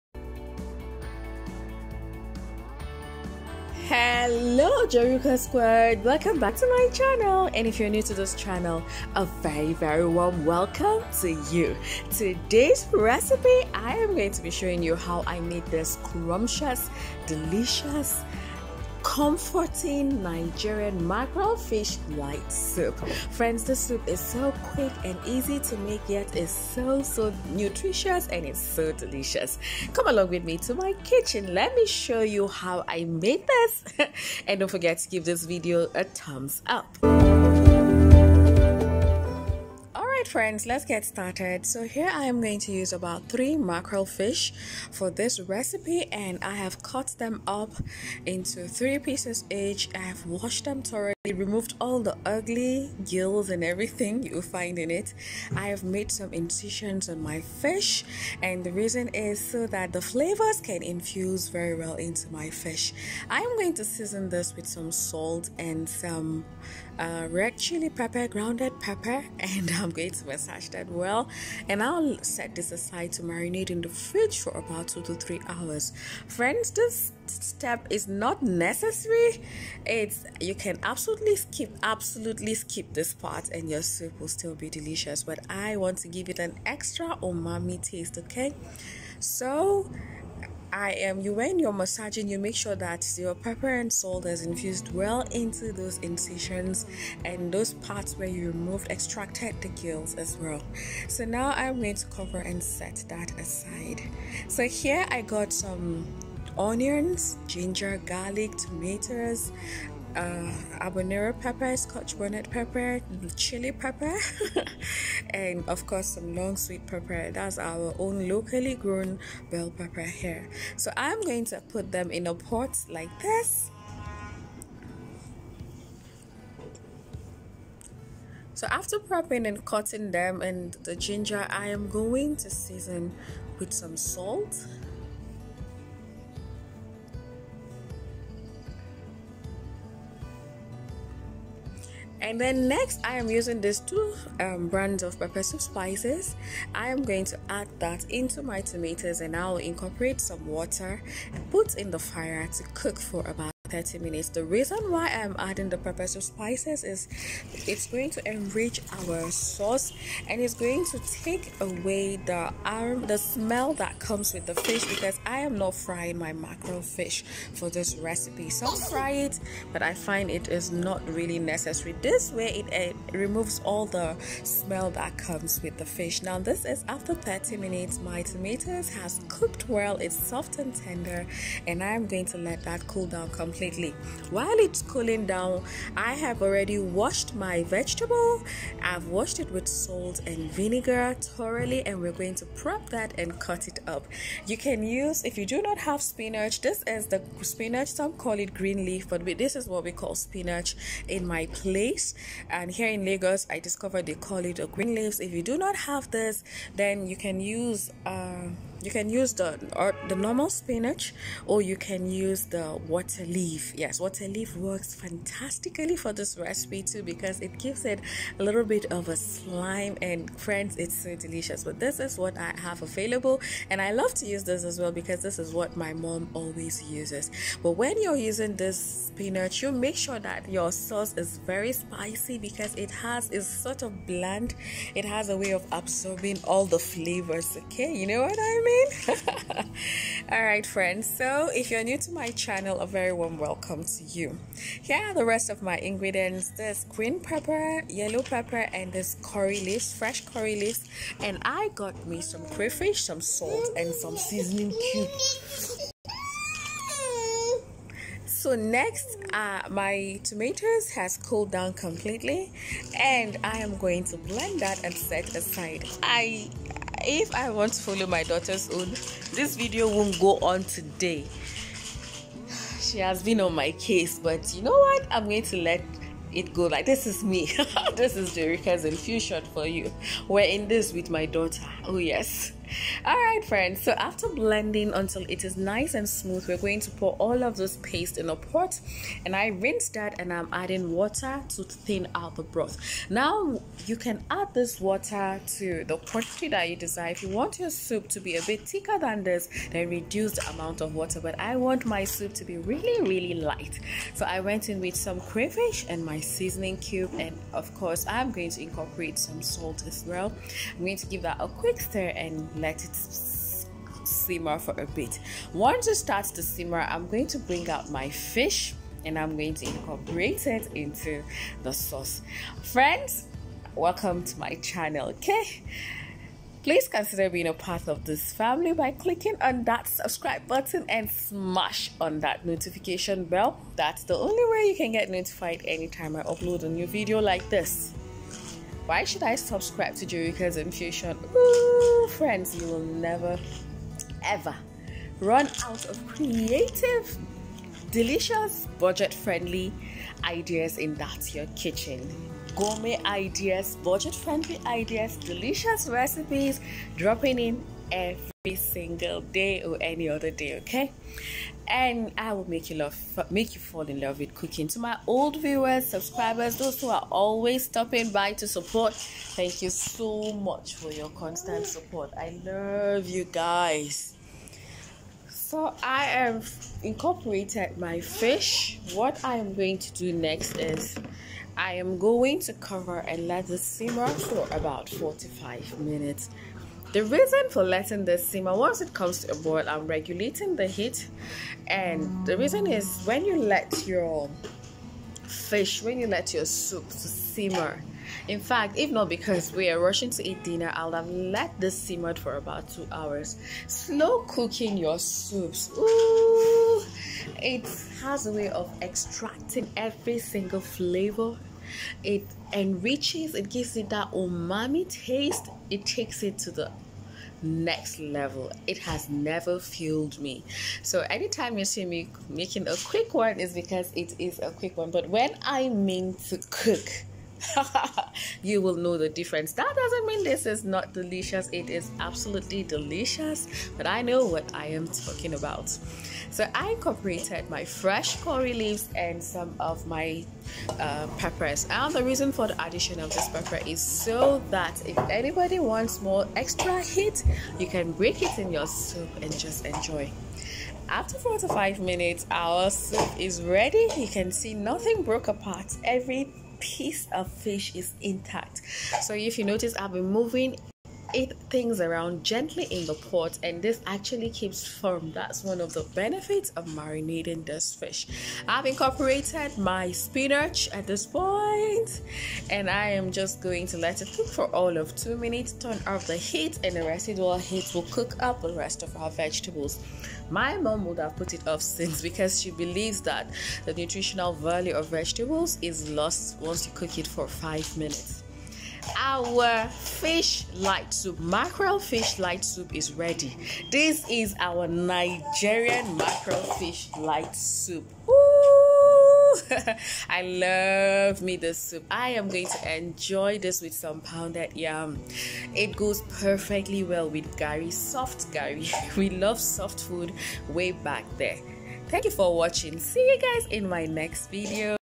HOW TO MAKE MACKEREL FISH LIGHT SOUP //MASA SOUP.. Tasty mackerel fish soup.
Mackerel Fish Light Soup. How to make Mackerel Fish  Light Soup..Tasty mackerel fish soup..

Although Mackerel fish light spinach soup is not popular in the Western and Eastern part of Nigeria, it is  much loved soup in the Northern and Middle- Belt Part of  Nigeria.
Spinach Fish light soup is enjoyed with a popular street food called masa and can be made with either beef bones,meat, cowfoot or fish...

Today i show you how to make the mackerel fish version of this much enjoyed delicay but with a Joroukas Infusion twist. This Mackere light fish spinach soup pairs well with pounded yam or Eba as well..
Bon Appettit.